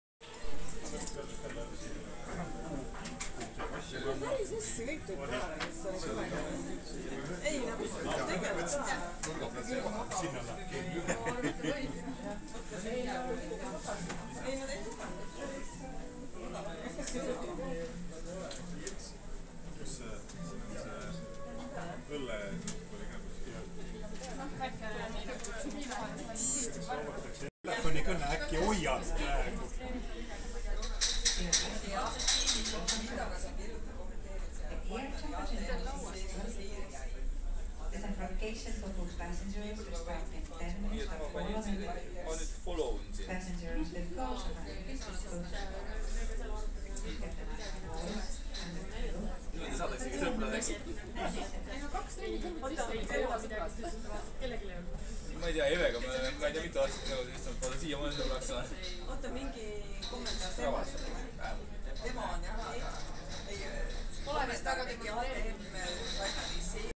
Поездка в Финляндию, Хельсинки. Погрузка в Таллинне. Прибываем в Хельсинки.
Часть 1.
Погрузка в Таллинне. Прибываем в Хельсинки.